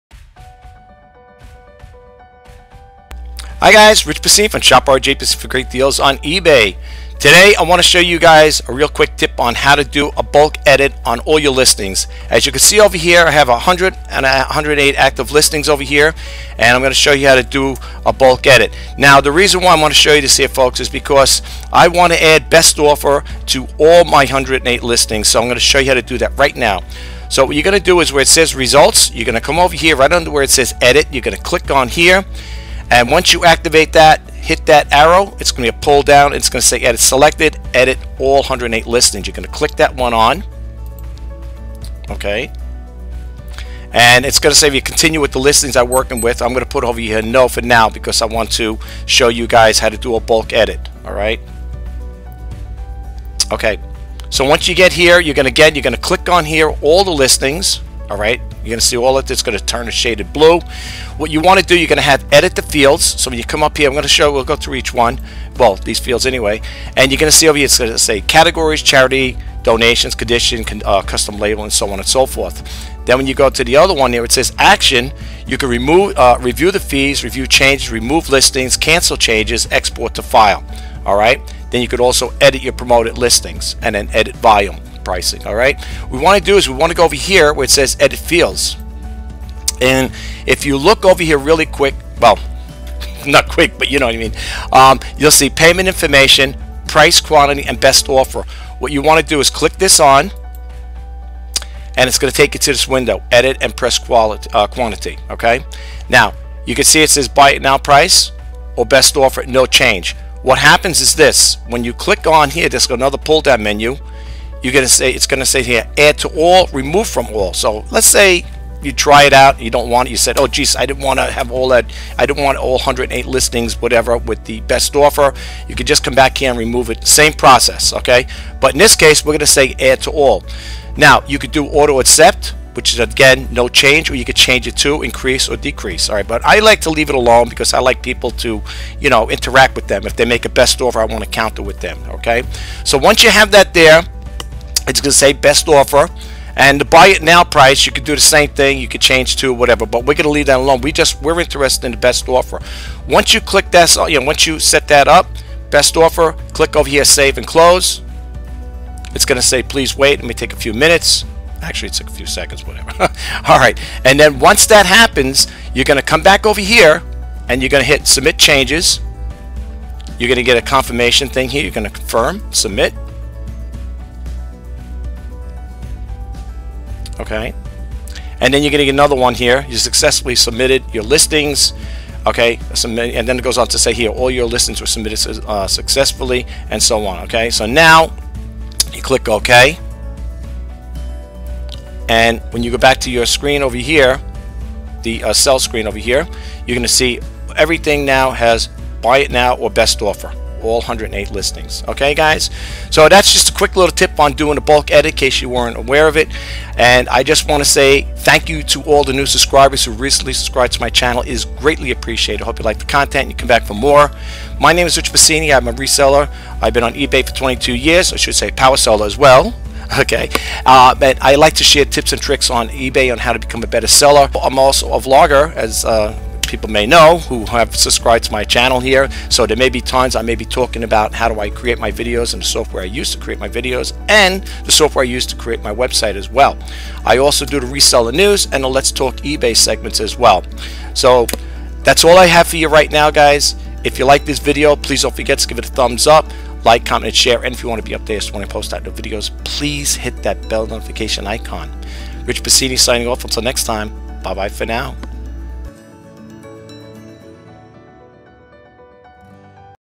Shop RJPASINI FOR GREAT DEALS ON EBAY 2-19-22
In this video, I go over how to do a bulk edit for your eBay listings.